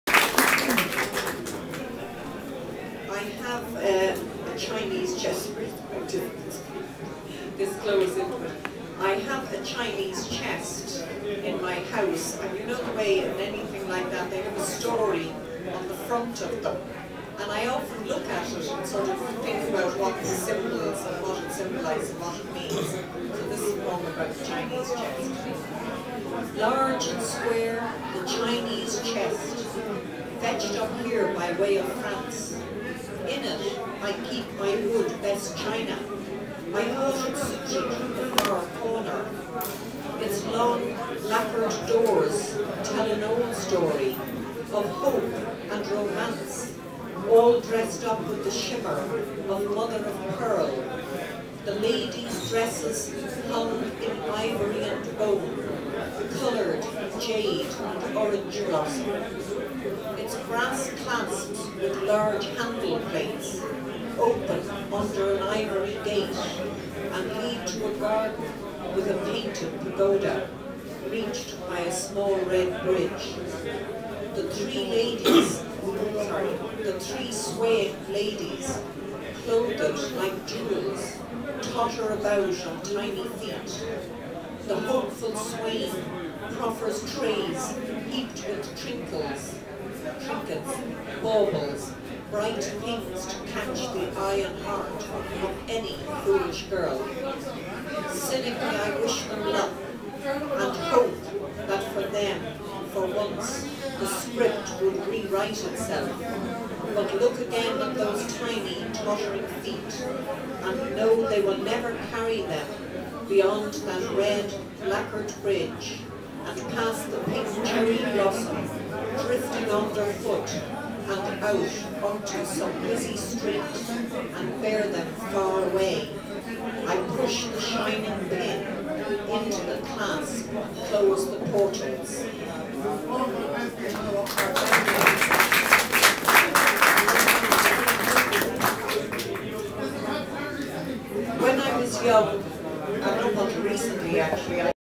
Jean O'Brien reading at the White House, Limerick, Ireland
Jean O'Brien is a Dubliner now living in the Irish Midlands. Her work is widely published
in magazines and journals. She has published two other collections, The Shadow Keeper,
(Salmon 1997) and Dangerous Dresses (Bradshaw Books, 2005) Her latest book,
Lovely Legs is recently published by Salmon. She holds an M.Phil. in Creative Writing
from Trinity College, Dublin. She facilitates creative writing classes for a wide variety
or organizations from the Irish Writers' Centre, Dublin City Council and other County
Councils to Mountjoy, Limerick and the Midlands Prisons.  She was Writer-in-Residence for Co. Laois in 2005. She was the 2008 recipient of the Fish International Poetry Award. Also in
2008 she was commissioned to write a poem for the Oxfam Calendar.
Her poetry was described by Fiona Sampson writing in the Irish Times as "...effortless writing, graceful and exact as any pirouette in its insight".